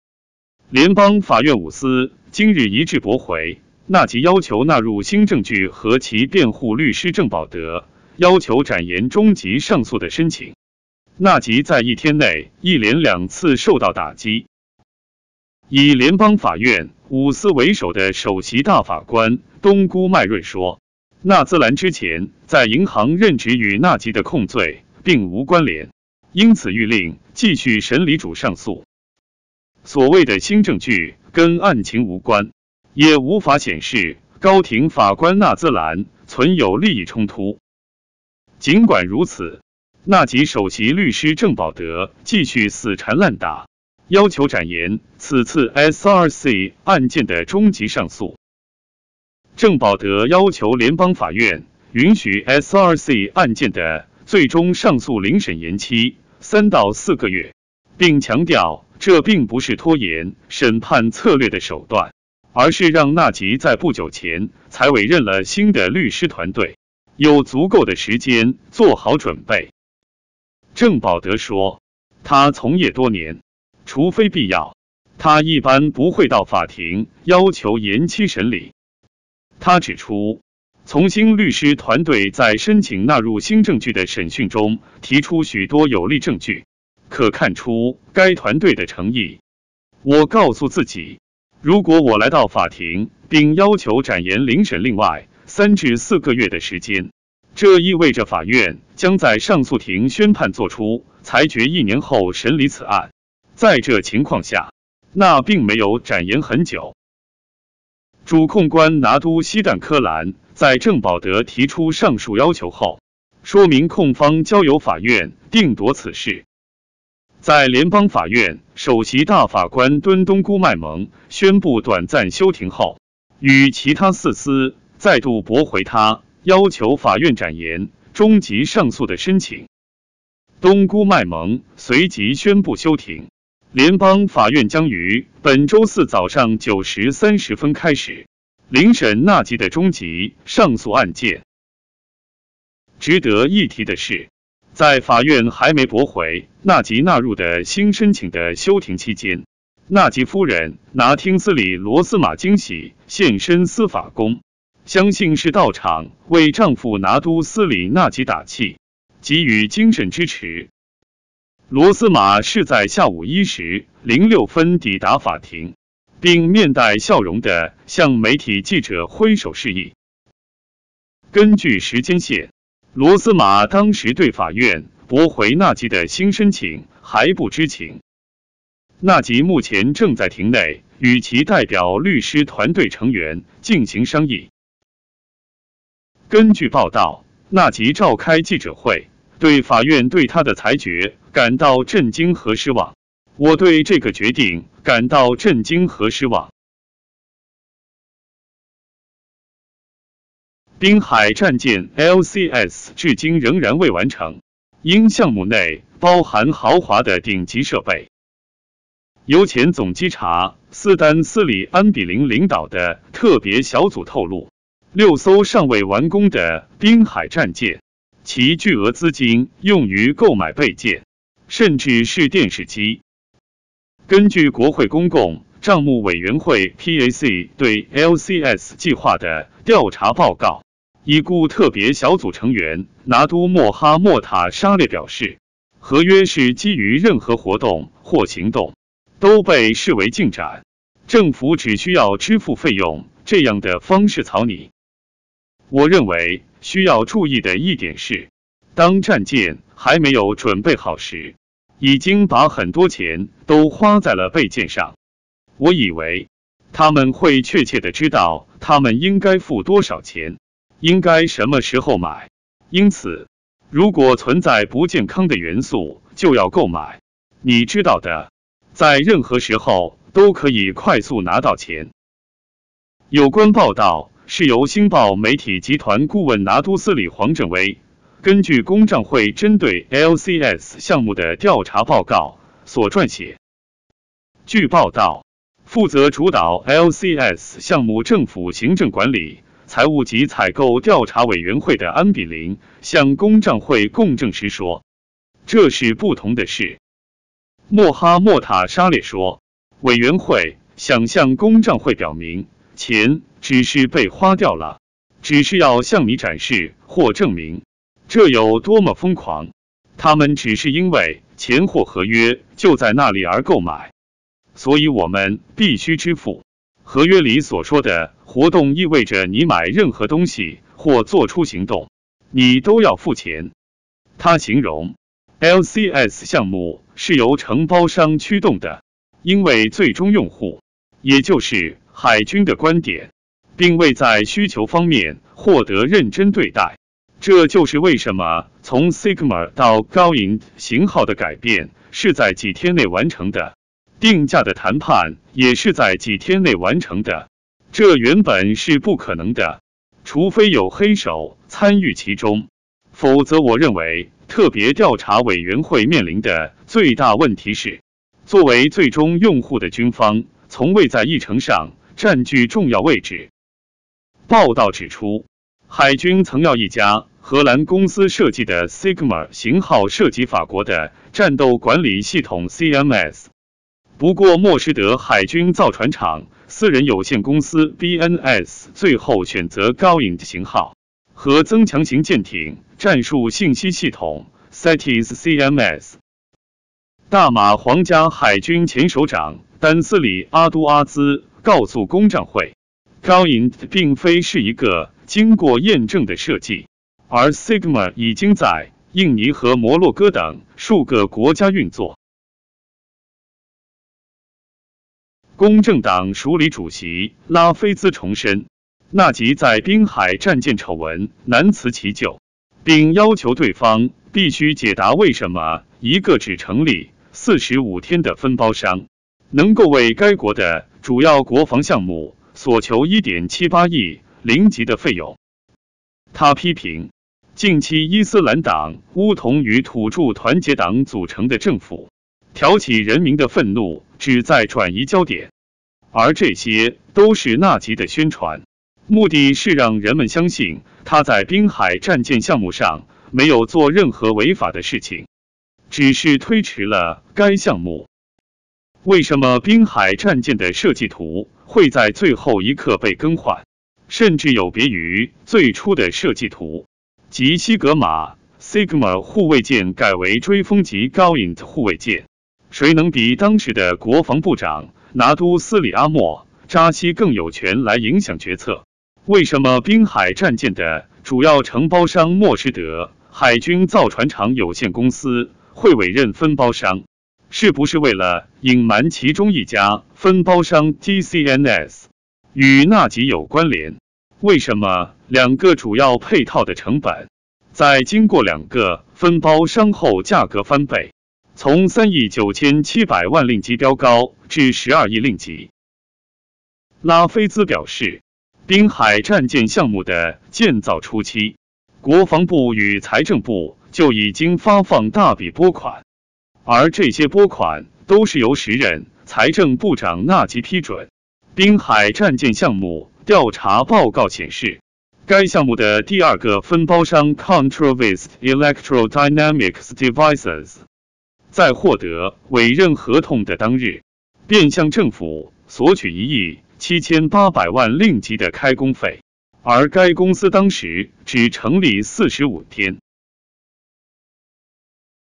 贪污无罪才是震惊！在法院耍花招一连两次被法官打脸，纳吉召开记者会作出回应
联邦法院五司今日一致驳回纳吉要求纳入新证据和其辩护律师郑宝德要求展延终极上诉的申请，
纳吉在一天内一连两次受到打击！
以联邦法院五司为首的首席大法官东姑麦润说，
纳兹兰之前在银行任职与纳吉的控罪并无关联，因此谕令继续审理主上诉。